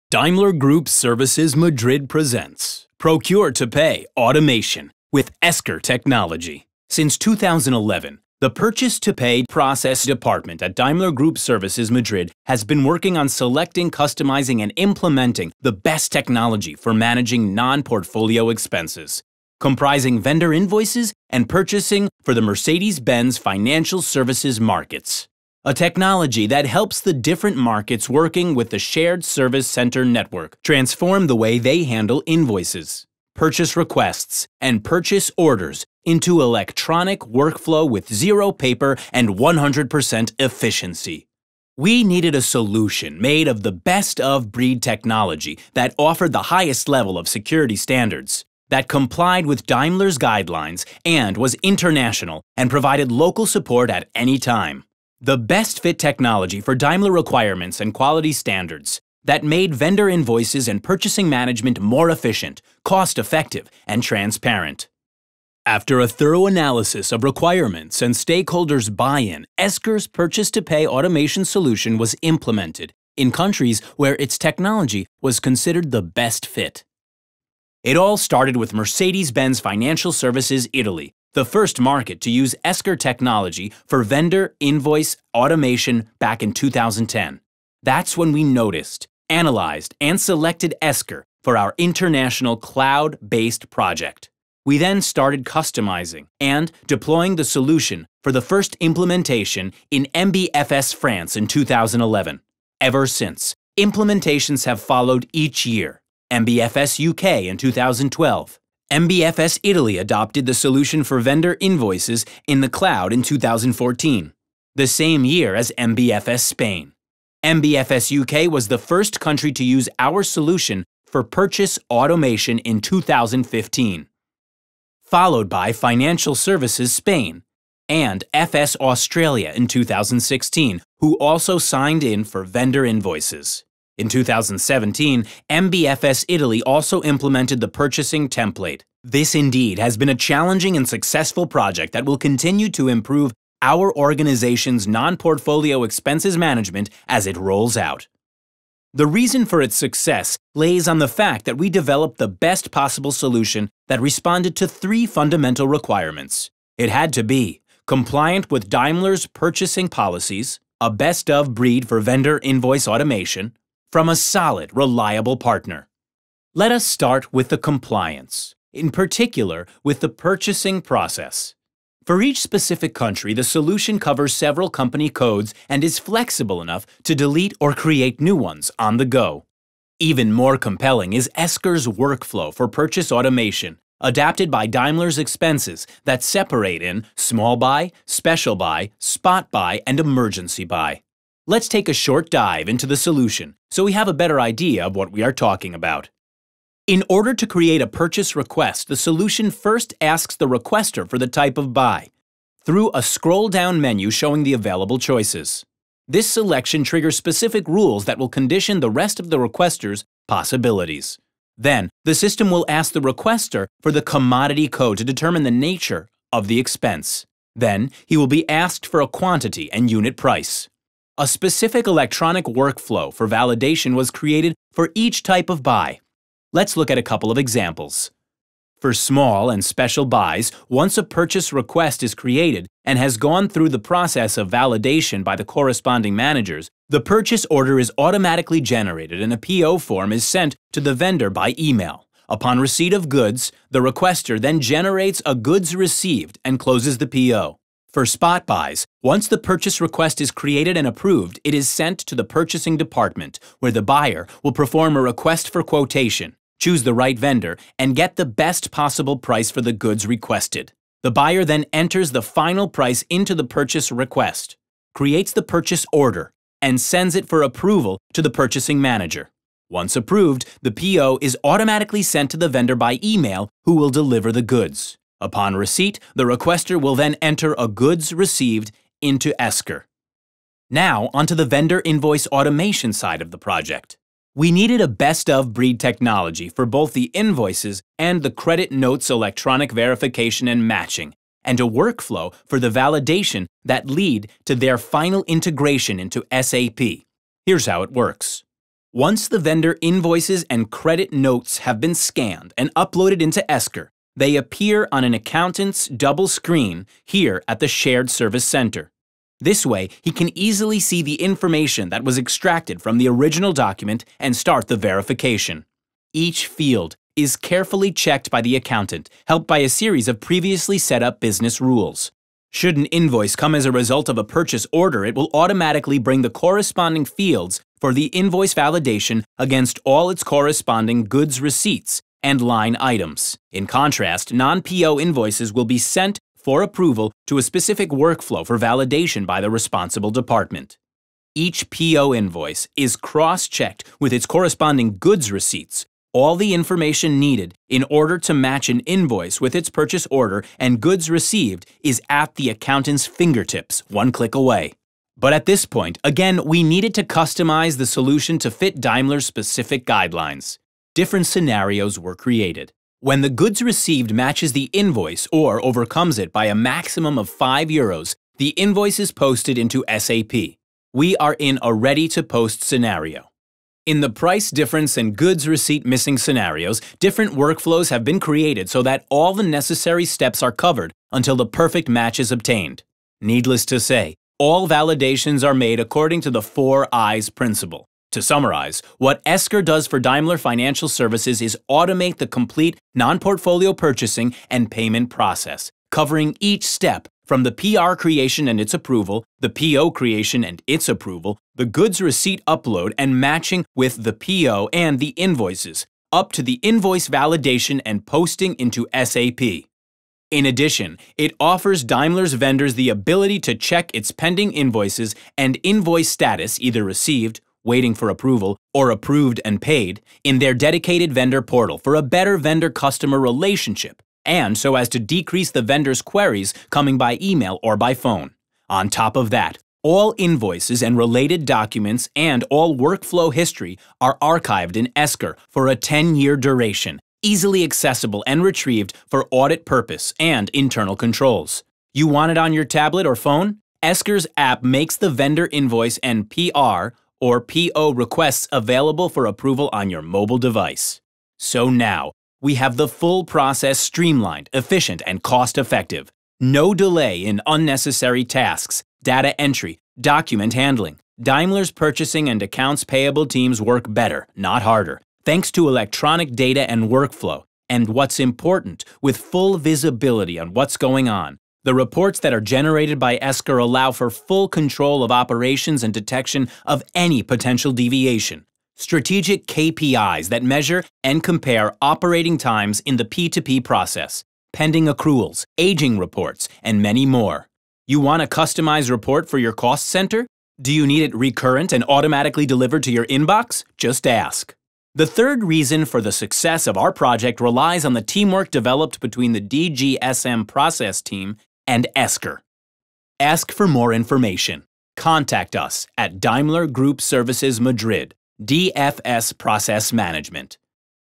Daimler Group Services presents: Procure-to-Pay automation with Esker
Daimler Group Services Madrid has teamed up with Esker to develop the best Solution for the Purchasing and Vendor Invoice Automation of non-portfolio expenses in those market where Esker was considered the best fit. This video explains the solution's rollout, history, and details on its customization and use at Daimle